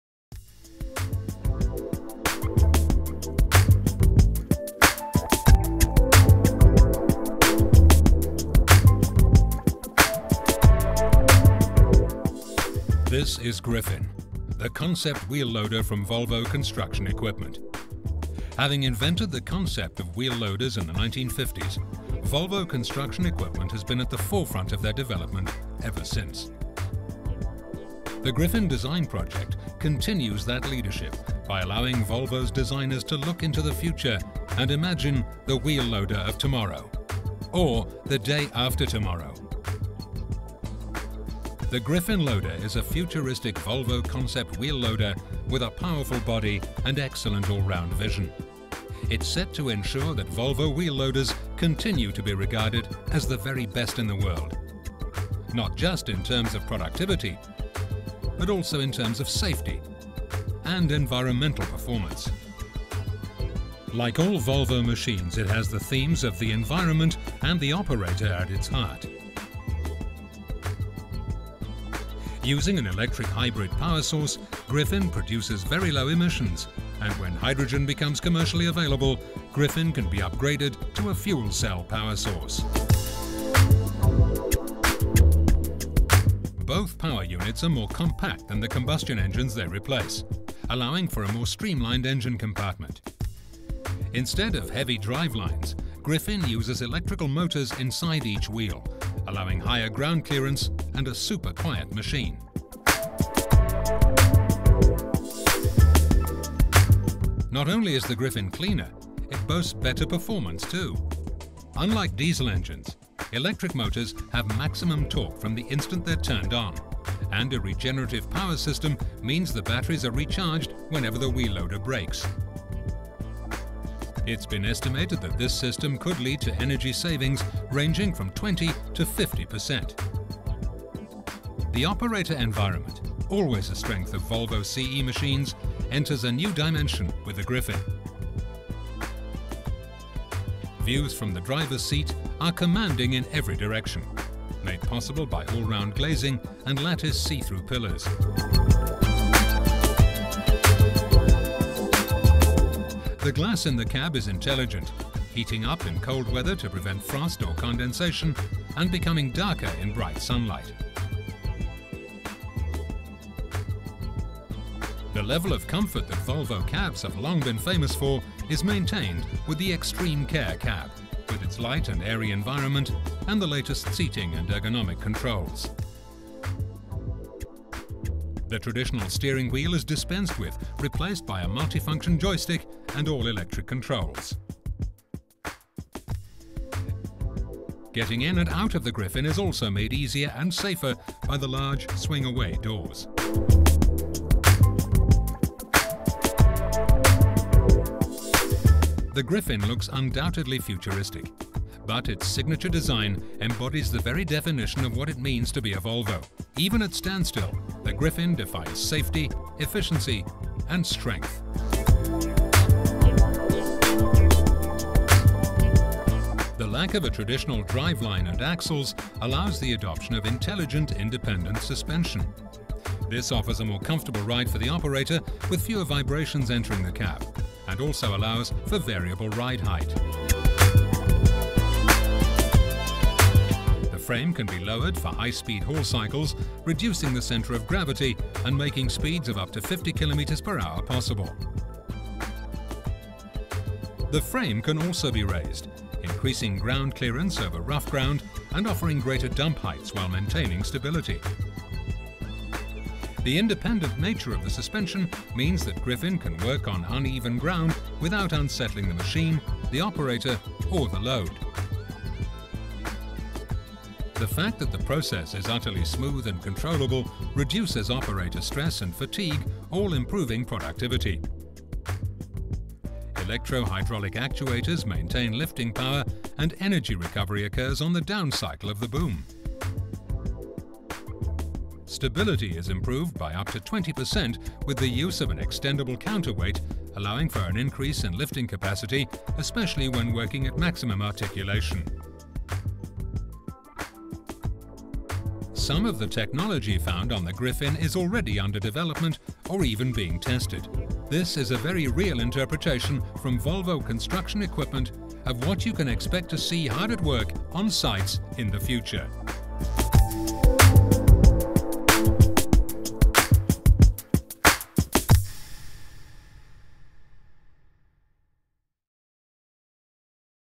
Volvo GRYPHIN - Volvo's futuristic Loader concept machine
Presentation of the GRYPHIN, possible Volvo evolution of the construction equipment (here a loader) in the years to come.